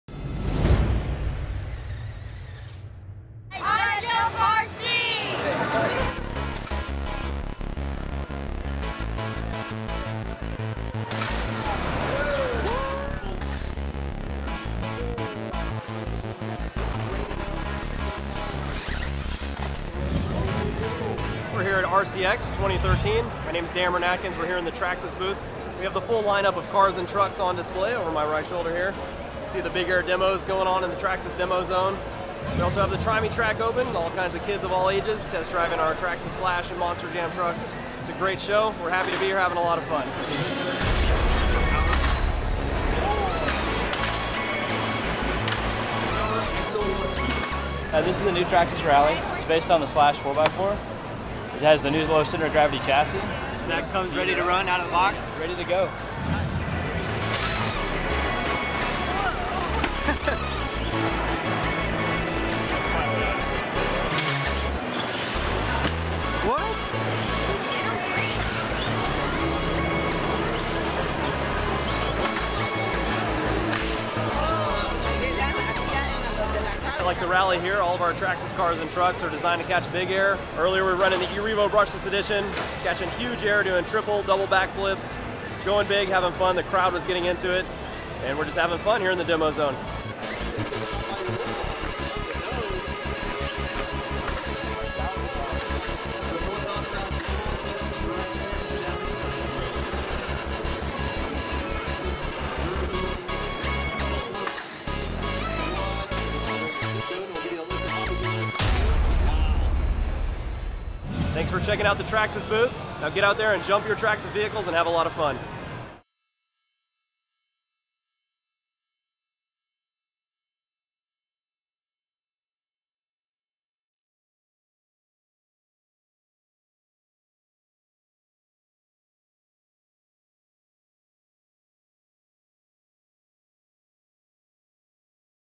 RCX 2013 Long Beach - Remote Controlled Car Expo - INSANE RC Car Jumping at Traxxas Booth
I spent a good chunk of my day at RCX 2013 (The RC Expo in Long Beach) at the Traxxas Booth.  Partly because I'm a Traxxas fan, mostly because they had some CRAZY Demo action going on!  I couldn't take my eyes of of their brand new Traxxas Rally and some of the sick tricks being thrown down by driver Scott Davis (2013 No Limit Freestyle World Champion).

VIDEO STATS
RC Cars: 1/10 Scale Traxxas Rally, Traxxas E-Revo Brushless, Slash 4X4
Driver: Scott Davis - Traxxas
Edited with: FCP 7
Special Thanks: Damron Atkins for the interview, and Traxxas for the awesome booth at RCX!